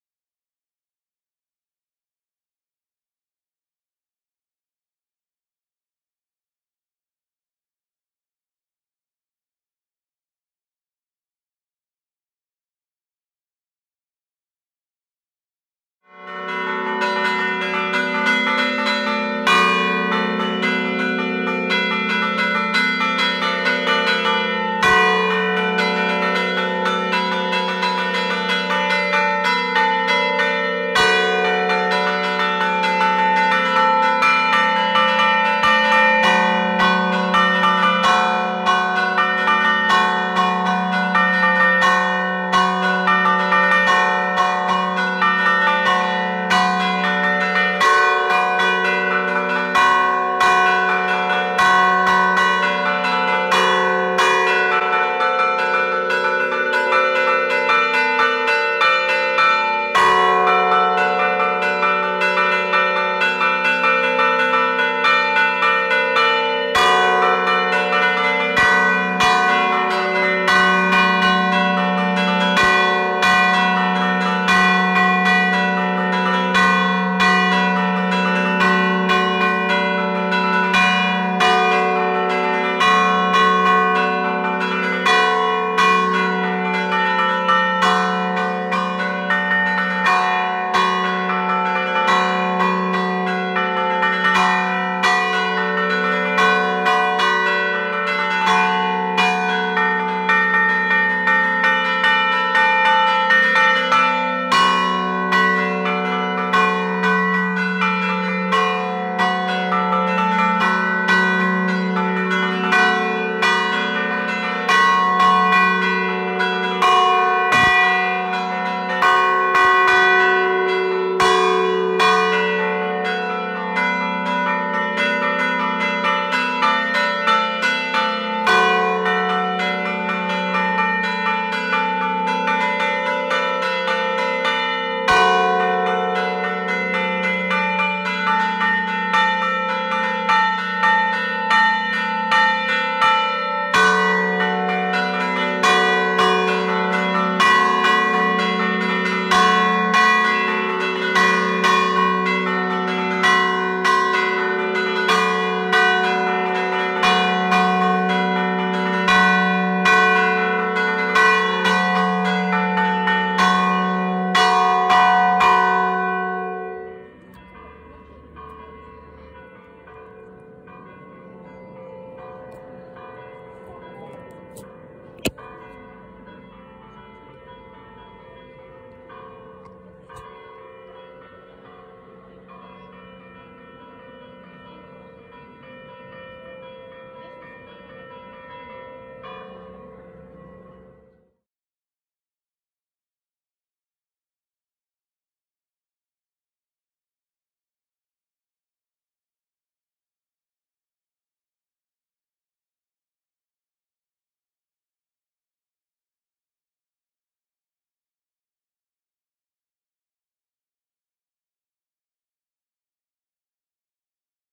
Le campane di Chiusavecchia (IM) - Battagliata eseguita dal campanaro locale
Campanari Imperiesi di un tempo, secondo video
Le campane di Chiusavecchia (IM)

Battagliata eseguita manualmente dal campanaro locale Giacomo Devia

3 campane in Fa#3, in accordo Tedeum (Fa#3-La3-Si3)
Campana maggiore: 1851 / VALLINO BRA
Campane mezzana e minore: 1820 / GIO BERTOLDO

Ringraziamo Giacomo che in occasione della festa patronale del paese ci ha dato l'opportunità di essere registrato per l'organizzazione. un Ringraziamento anche al parroco Don Klaus Per la disponibilità.